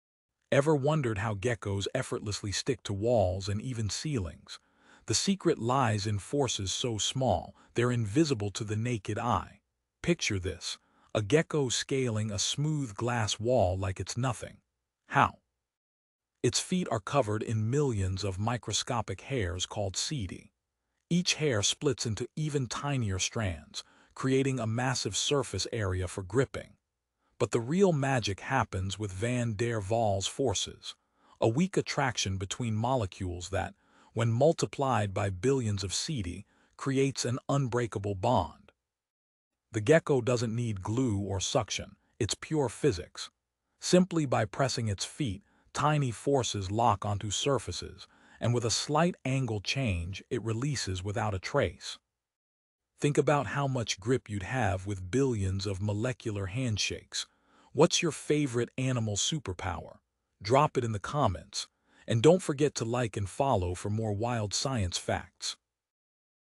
The Amazing Science Behind How Geckos Stick to Walls
Discover how geckos defy gravity with van der Waals forces and millions of tiny hairs!